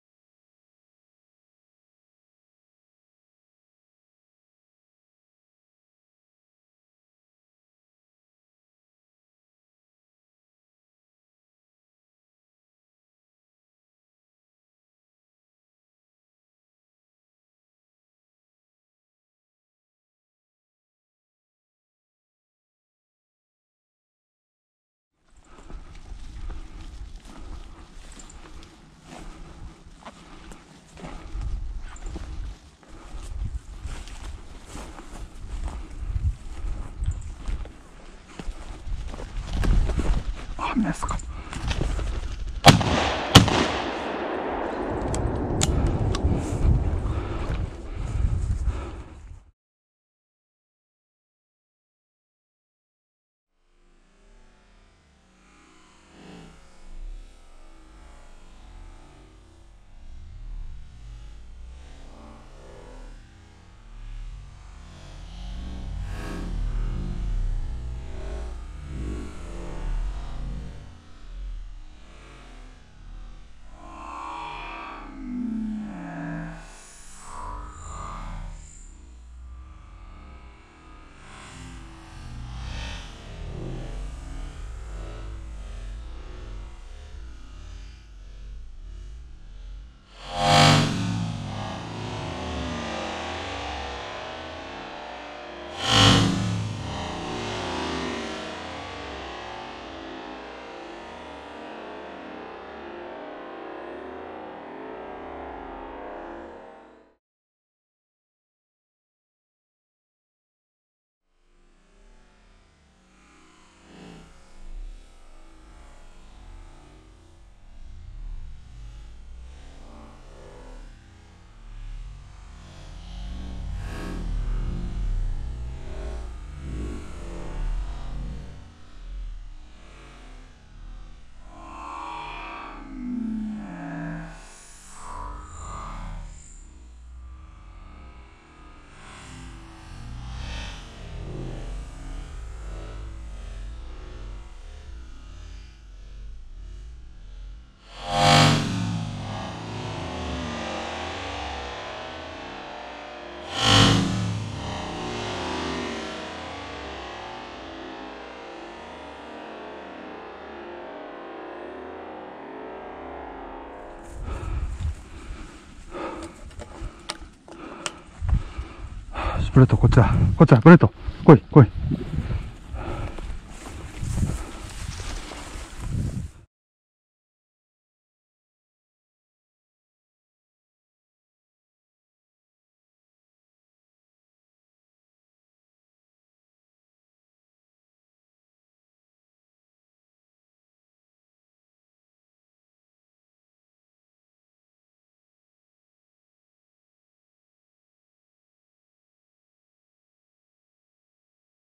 【ミニチュアダックスフンド】令和3年度狩猟での射撃・シーズン2羽目の雉
猟犬ミニチュアダックスフンドのブレッドでの狩猟、その射撃シーンを抜粋しました！
今回はシーズン2羽目の雉。
まさかのミニチュアダックスフンドのポイントからのスタートです。
最初に出たのは雌雉で見送りました。
次に飛び出したのは雄！
矢先は見通しが良かったのでススキの影に入る前にすかさず初矢を撃ち込みましたが見事に失中、二の矢は落ち着いて狙い直しました。
初矢は少し上を撃っている感じでした。
雉、小綬鶏は水平方向に飛ぶので見通しがきかなかったり危険と判断した場合には撃たない事が重要です。